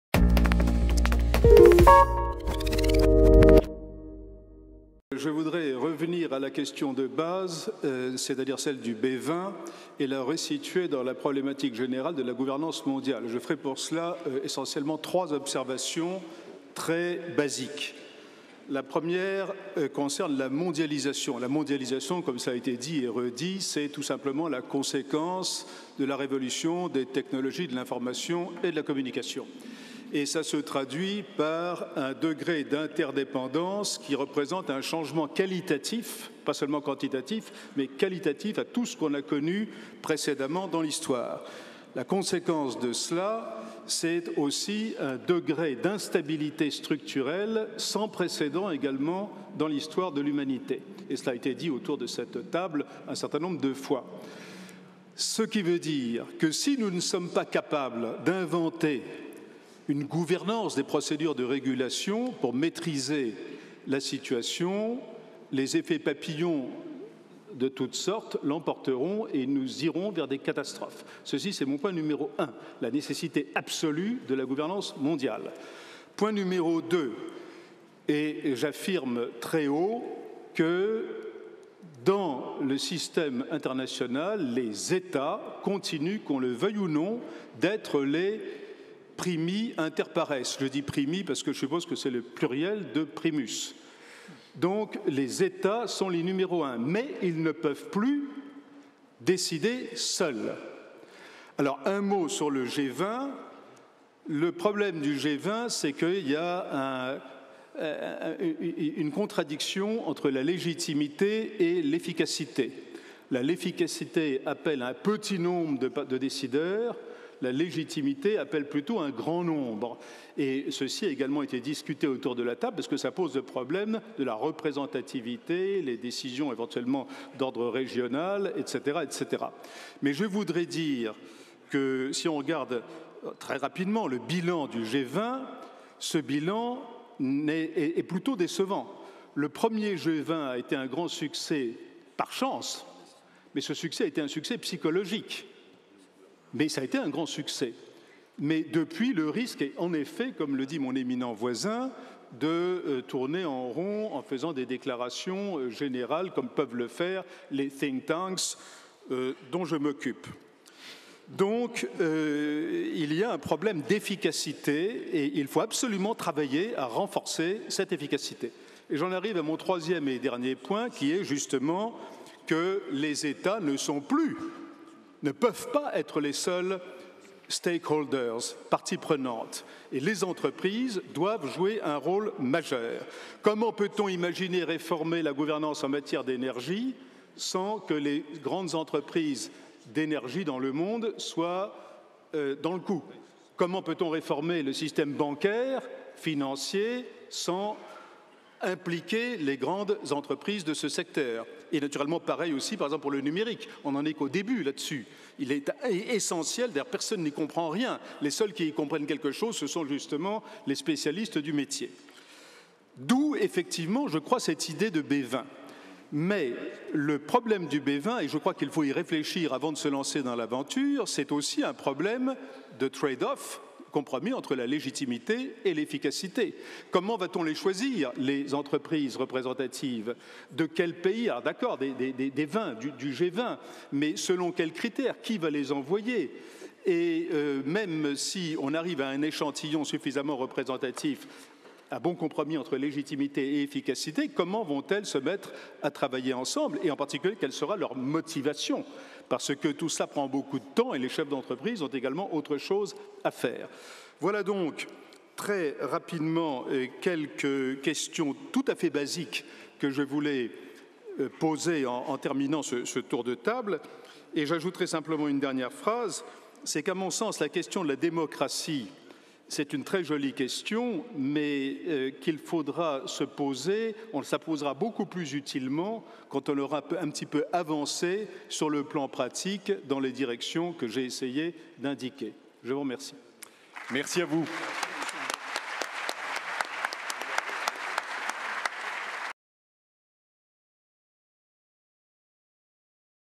Thierry de Montbrial: ''Les entreprises doivent jouer un rôle dans la réforme de la gouvernance''
Thierry de Montbrial est Directeur général de l'IFRI, l'Institut Français des Relations Internantionales.
il est intervenu le 31 août 2011 lors de l'Université d'été du MEDEF, dans le cadre de la séance plénière "Que j'aime ta couleur B20". Lors de ce débat, il a souligné "l'absolue nécessité d'une gouvernance mondiale". Il a également indiqué que les états ne pouvaient plus être les seules parties prenantes. Pour lui, il est nécessaire que les entreprises soient elles-aussi parties prenantes, comme c'est le cas avec le B20.